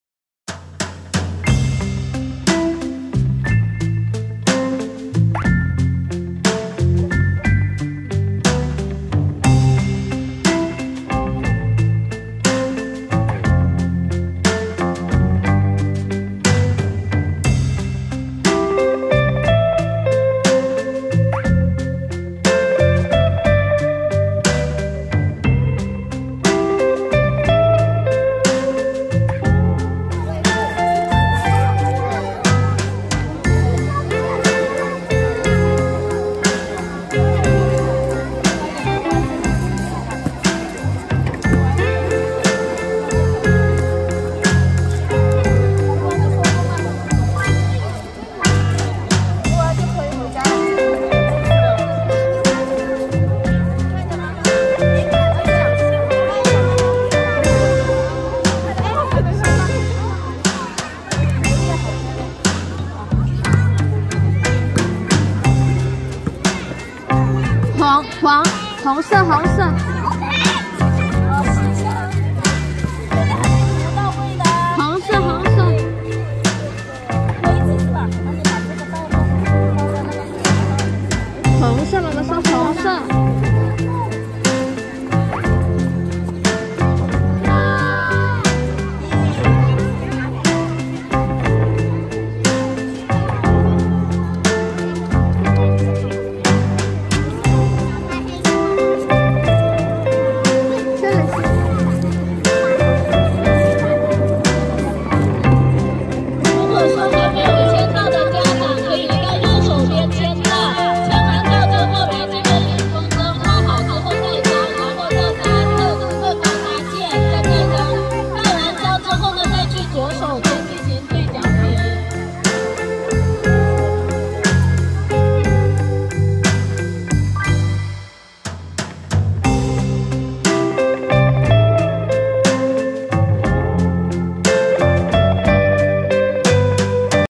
Pertama kali ikut kegiatan orang tua dan anak di Shenzhen | Keluarga Indo China
Hi Qing ai de Pengyoumen
Apa kabar semua ? Pasti semua ada bae-bae jo to^^

Pengyoumen..ini video pertama kali saya ikut kegiatan orang tua dan anak di Shenzhen. At that moment, papanya Guo masih kerja, so just Guo and mama. Bersyukur sekali mama sudah bisa sedikit speak mandarin Guo, jadi kita bisa ikut kegiatan ini.

Terima kasih sudah mau menonton video ini^^
Xie xie nimen
Ai ni e